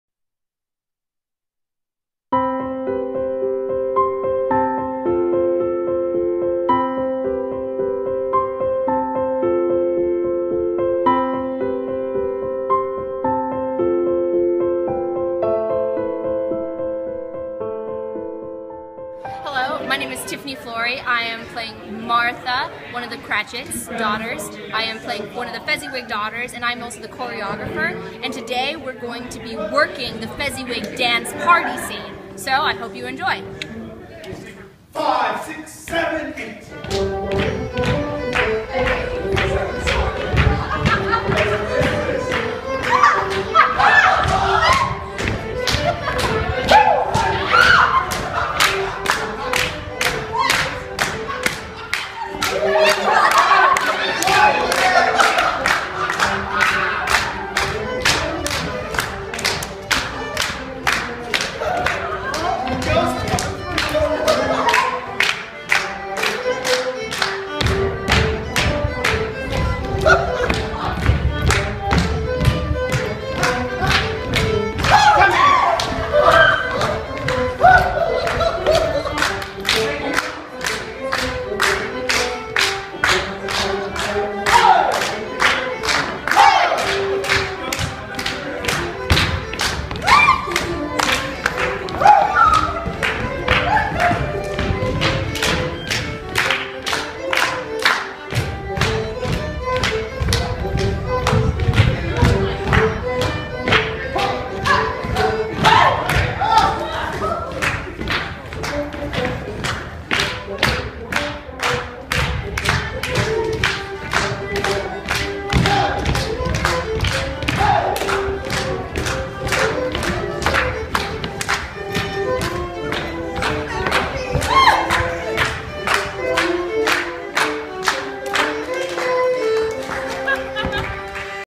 Fezziwig Dance
Actor/choreographer Tiffany Flory leads a rehearsal of the Fezziwig dance for the upcoming production of UI's A Christmas Carol.